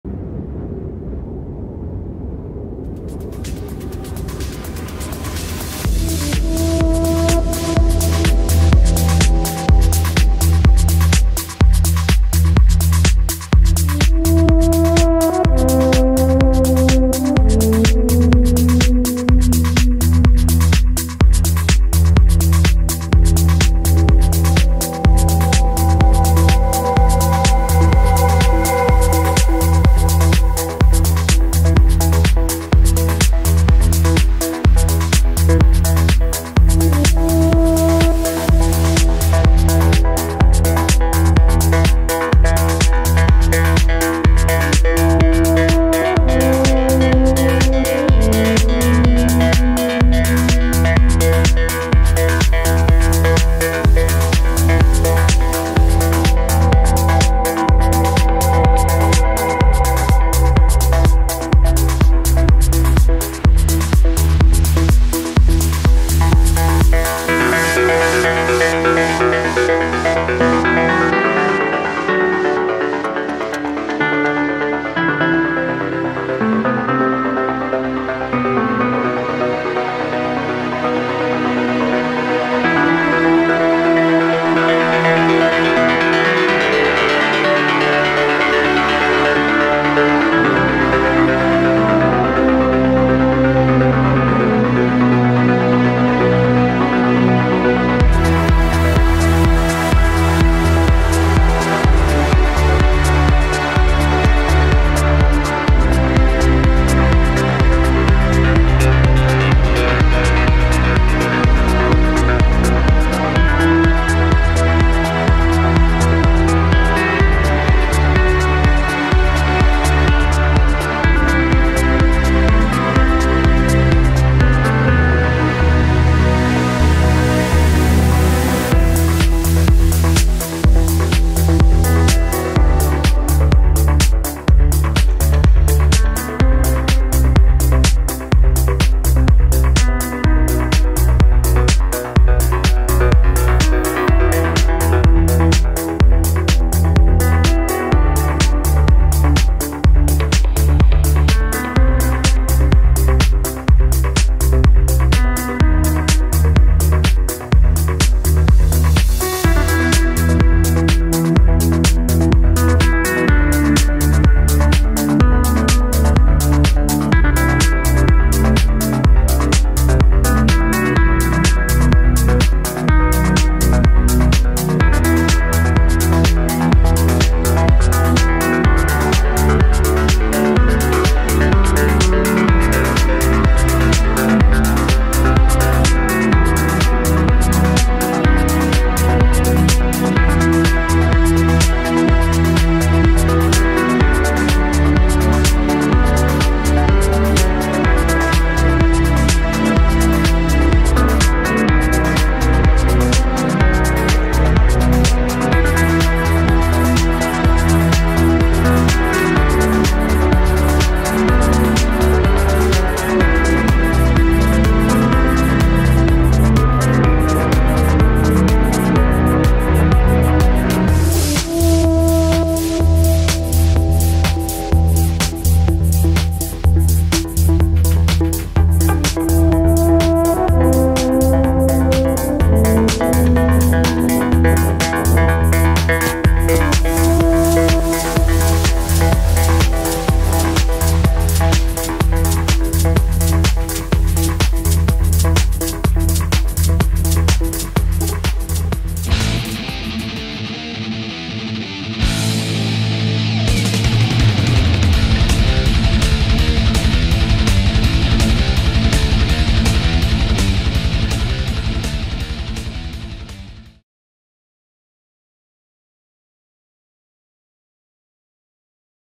Frosolone col Drone
Una glaciale giornata di sole a colle Acqua Spruzza, Frosolone